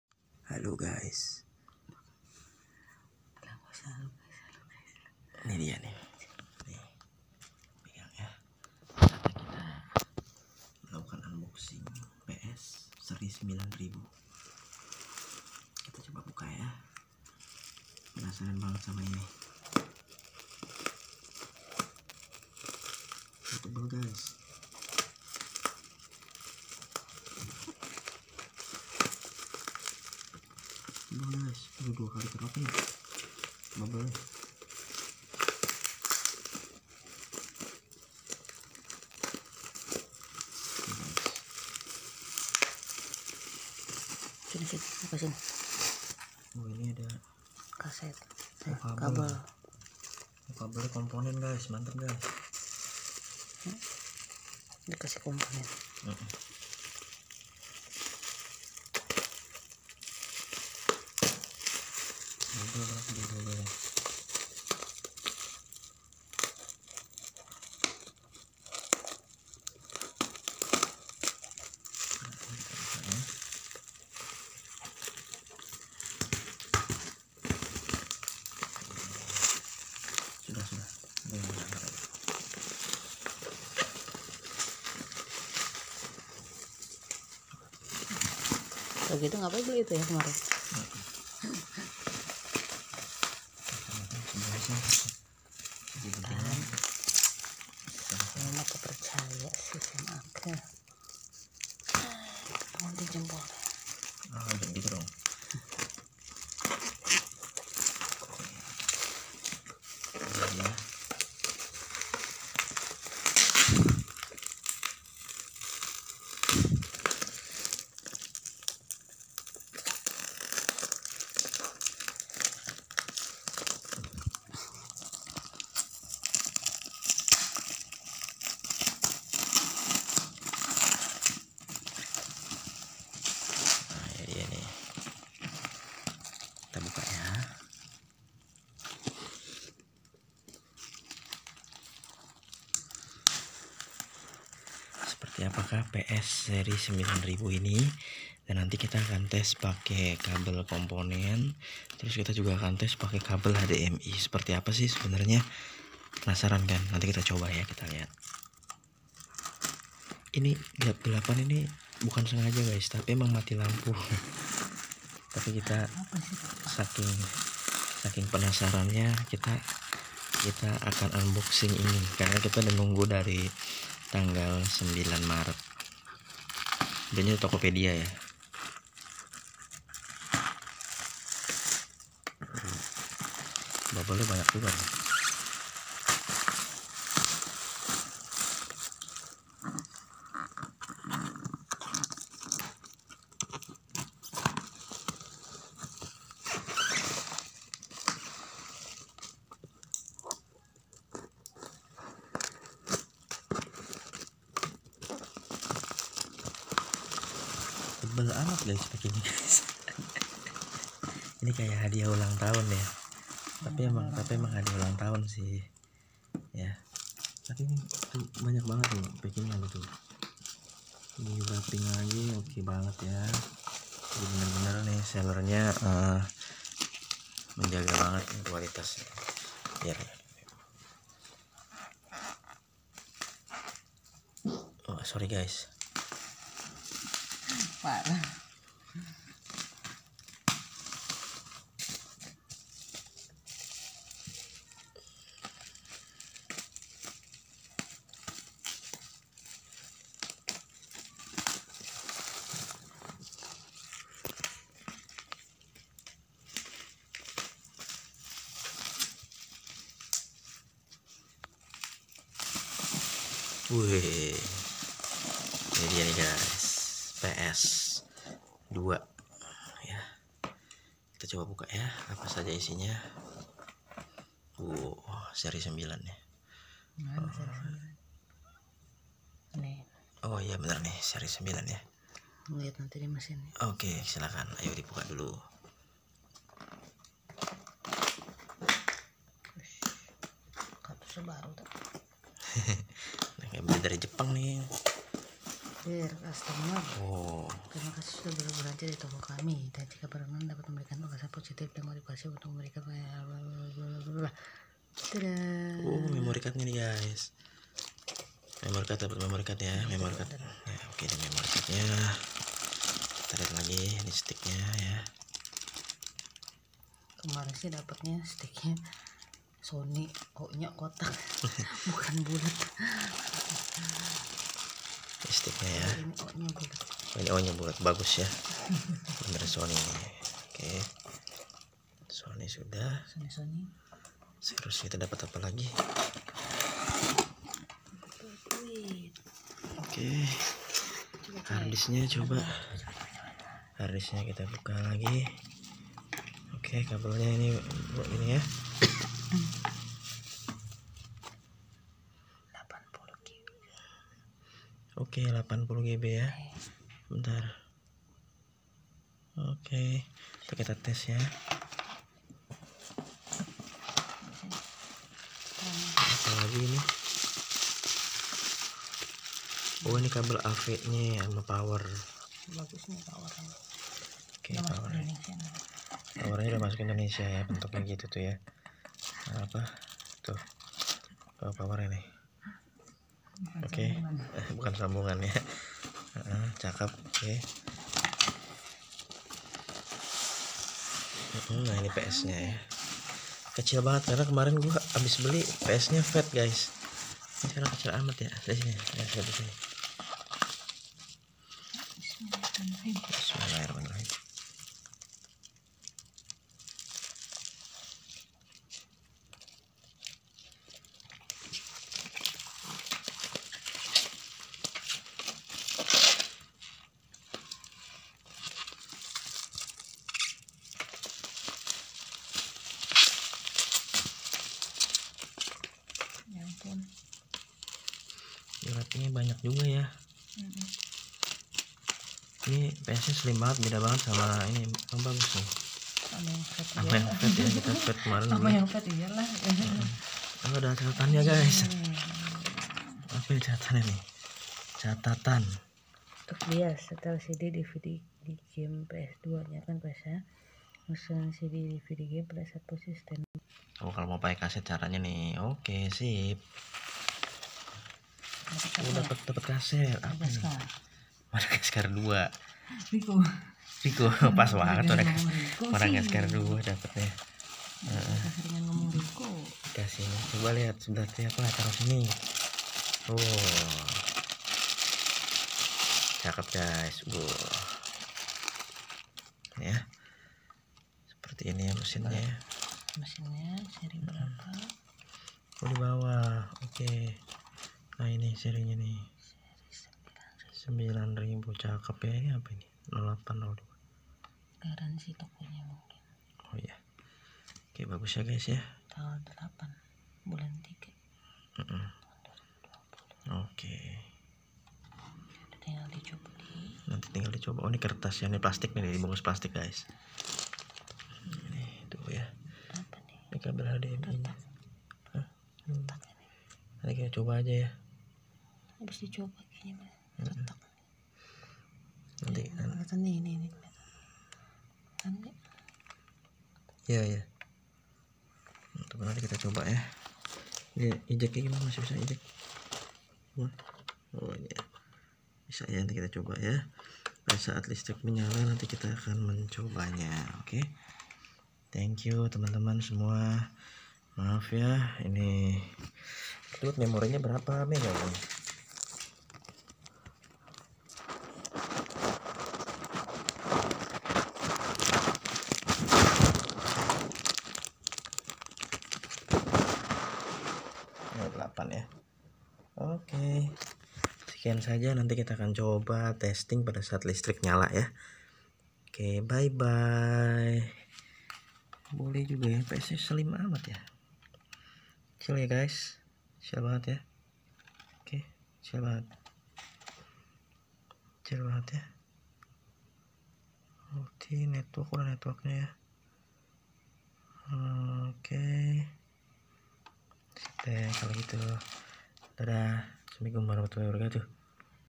PS2 Slim 90006 Unboxing Beli dari Tokopedia 2020
Baru beli PS2 dari Tokopedia hanya mau berbagi informasi